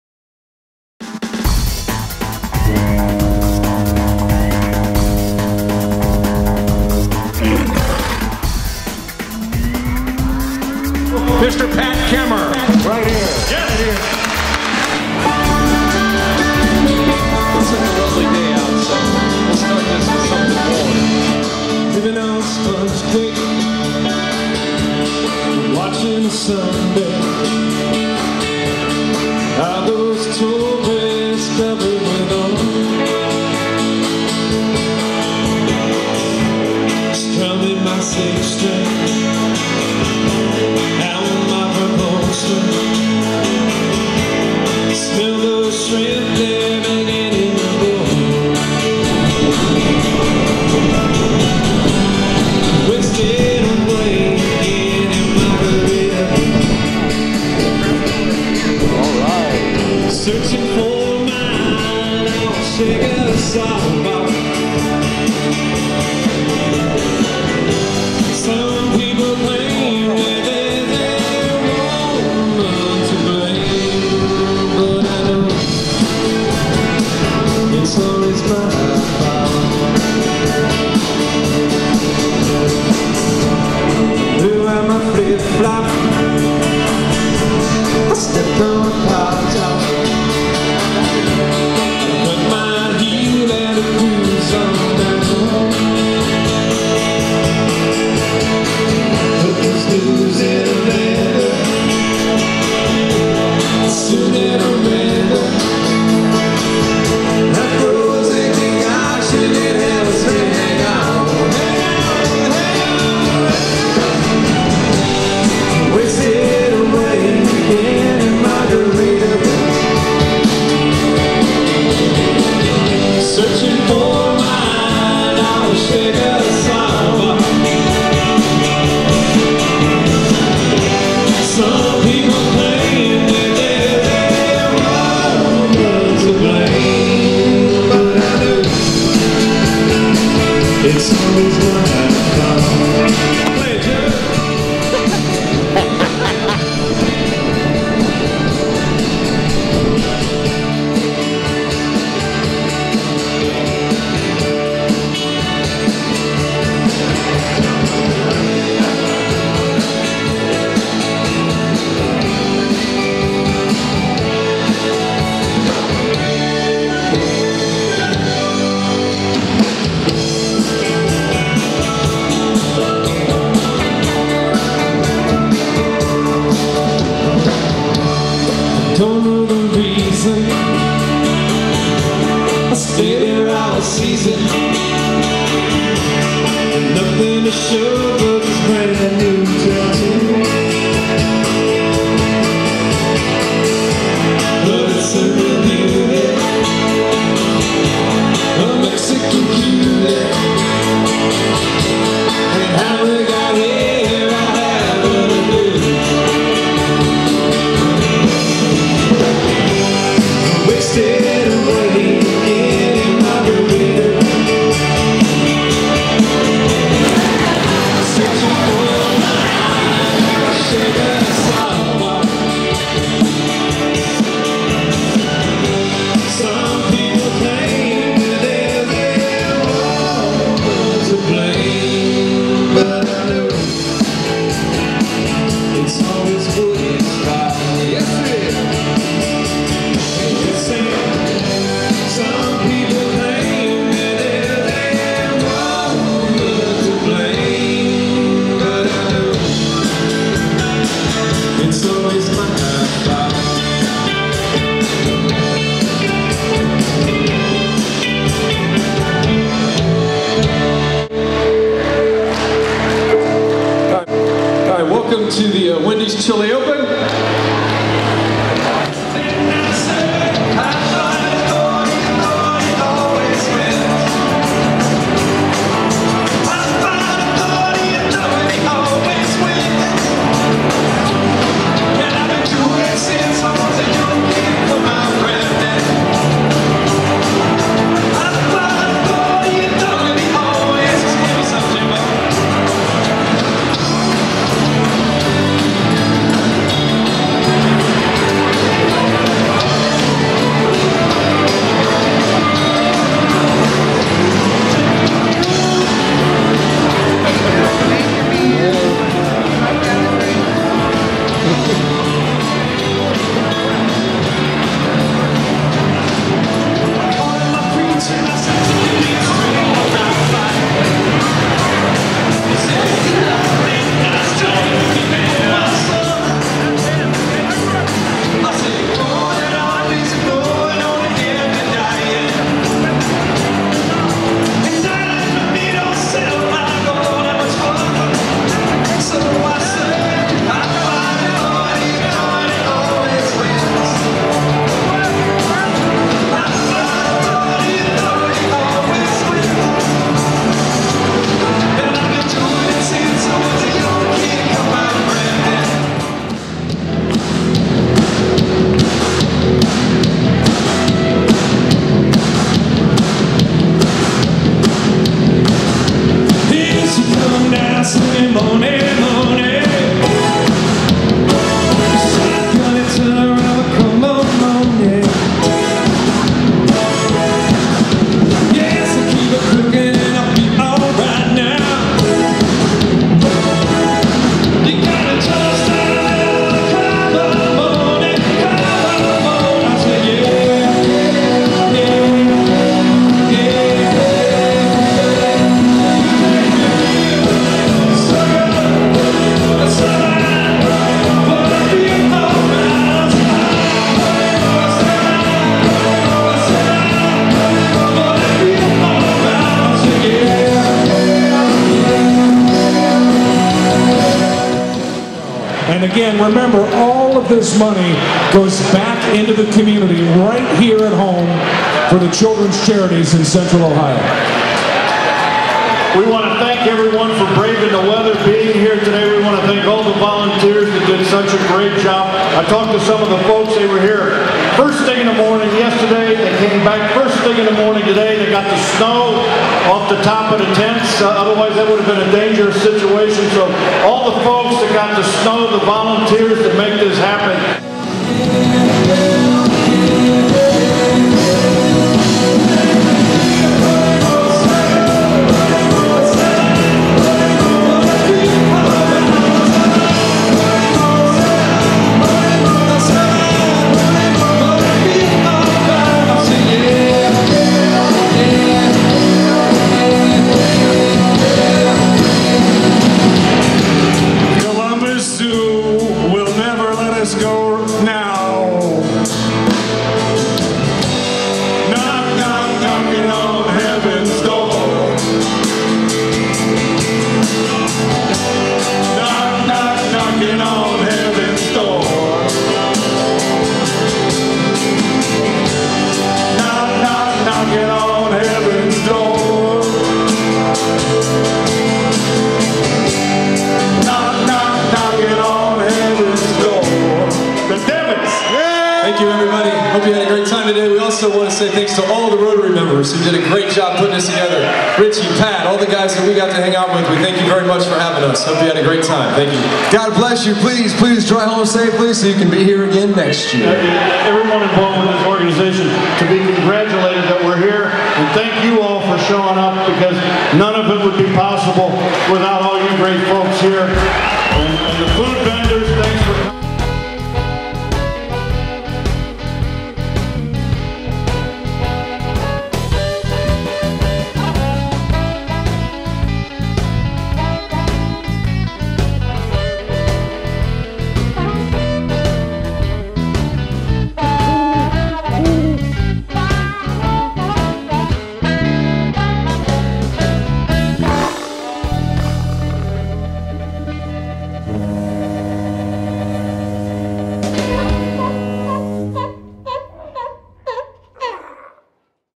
Wendy's Chili Open - Photo 6-FEB-2010.mov
The WENDY'S CHILI OPEN is coming back to the COLUMBUS ZOO on 5-FEB-2011.
This is easily the Wildest Party of the Year!     Call all of your friends and hitch up your Sleighs for the Columbus Zoo  at high noon on February 5, 2011.
This event is the Signature Event of the Westerville Sunrise Rotary.